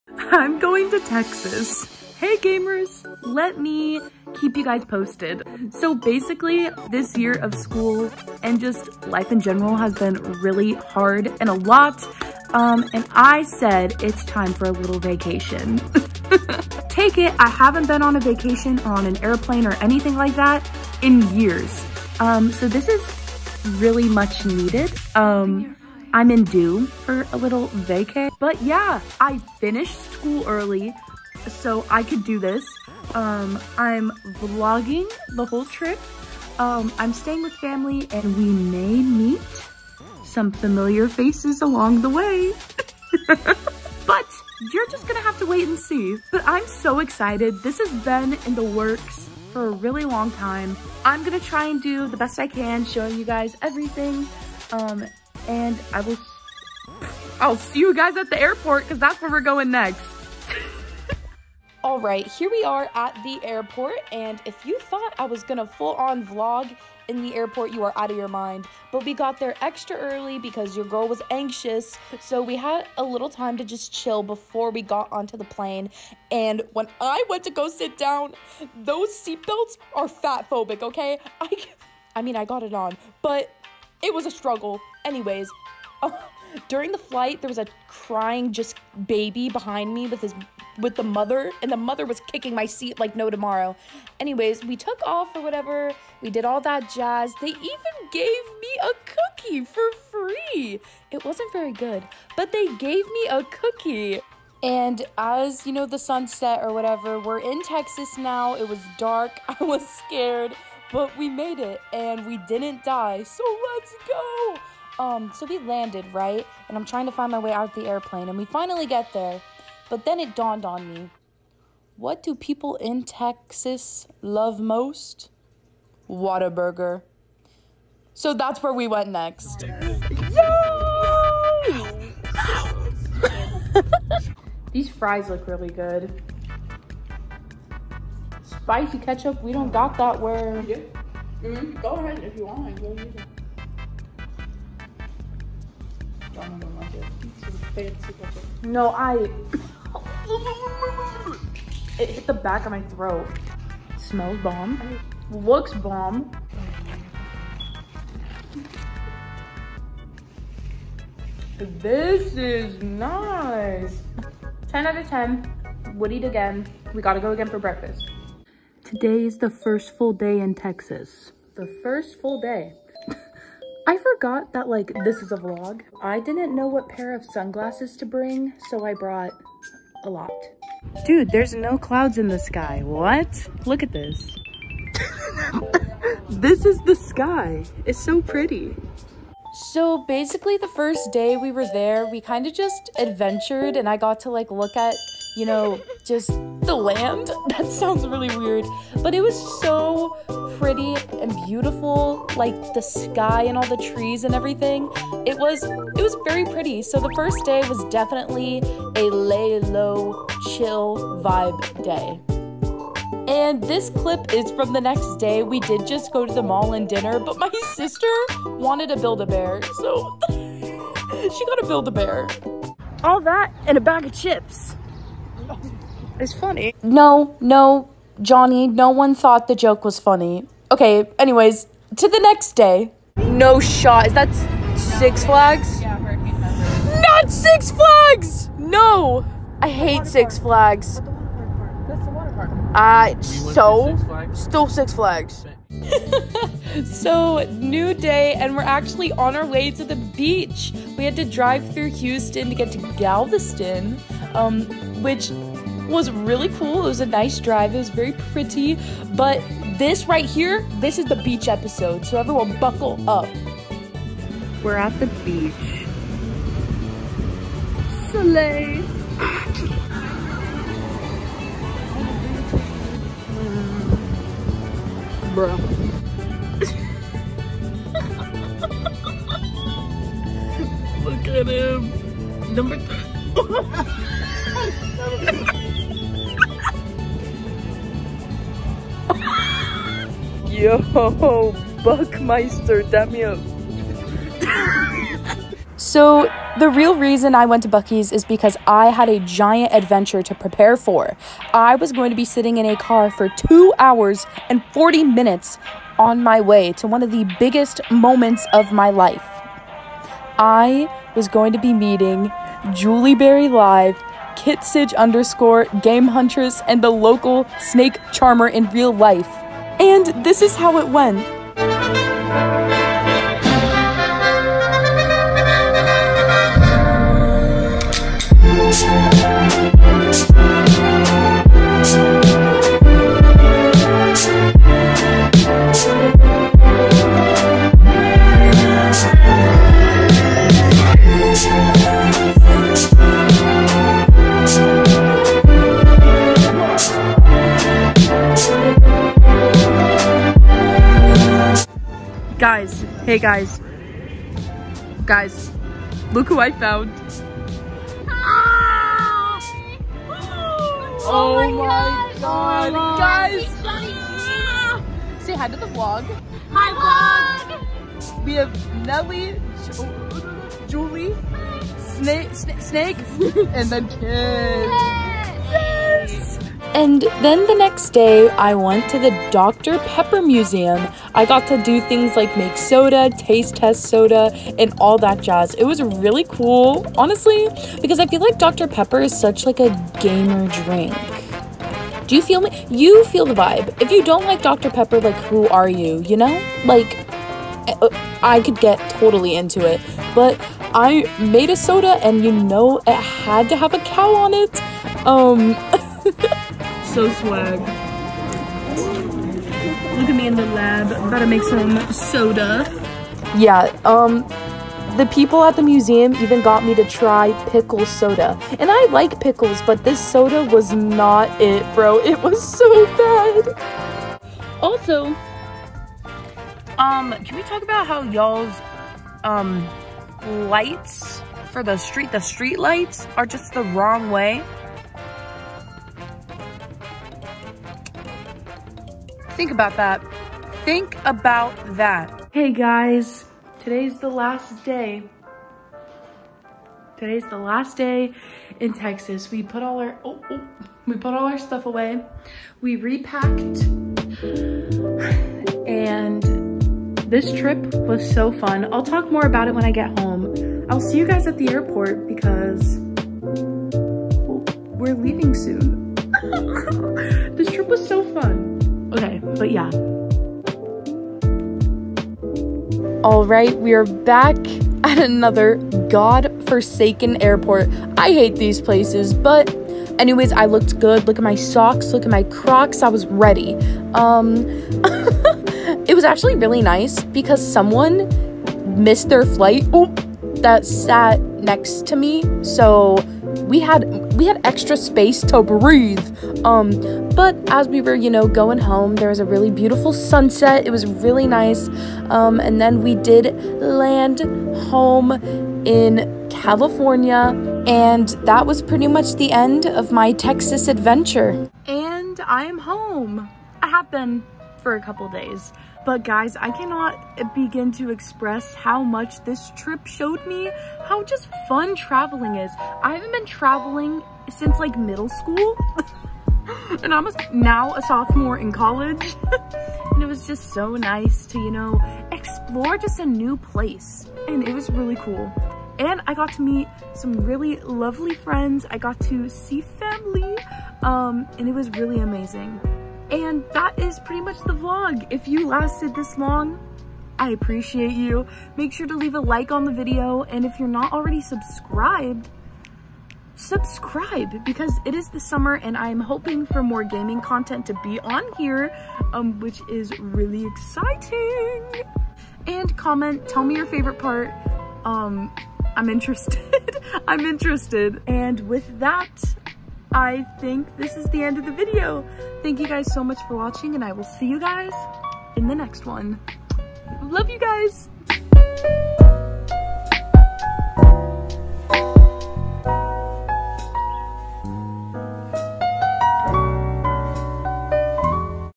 Jonni Makes Texas 1000% Funnier!
I WENT TO TEXAS🤠! In this vlog, Jonni and her sister go to Texas to visit family, along with some AMAZING FRIENDS!